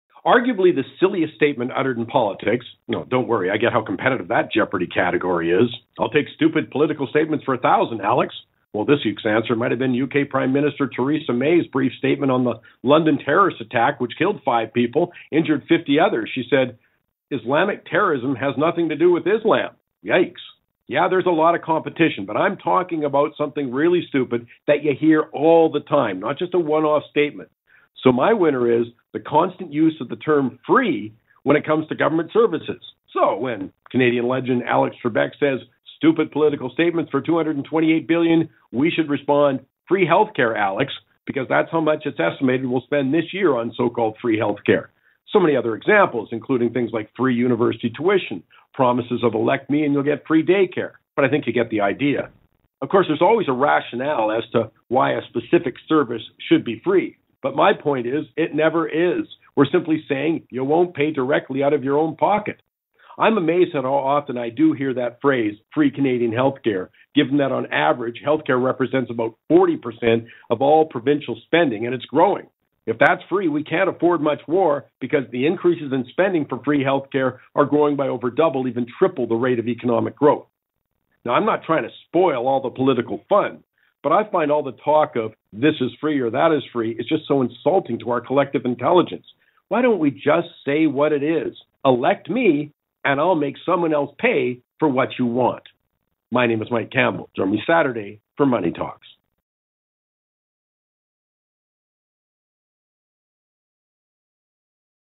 The Dumbest Word in Politics..."Free"!
Given that so-called "Free Healthcare" consumes over 40% of provincial budgets, it is hardly free. Indeed, the rate of growth in healthcare spending is triple economic growth. Its time to call it for what it really is.